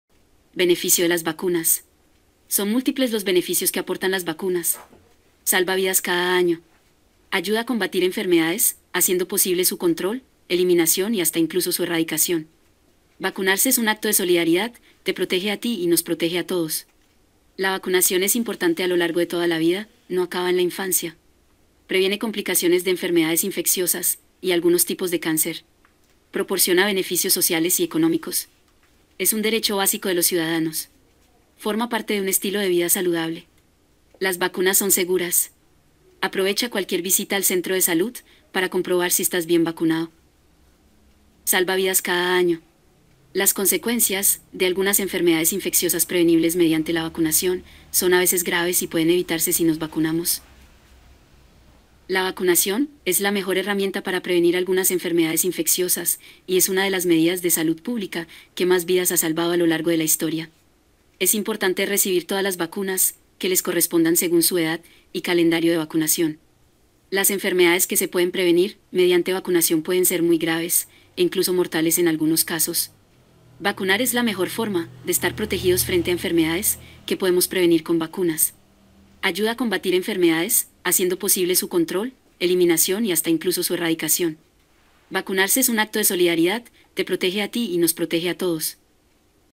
🔴 Beneficios DE LAS VACUNAS Para La Salud ✅ Porque USAR LAS VACUNAS
Bienvenidos a nuestro canal Beneficios Del, en este video te mostraremos los benéficos DE LAS VACUNAS  para la salud.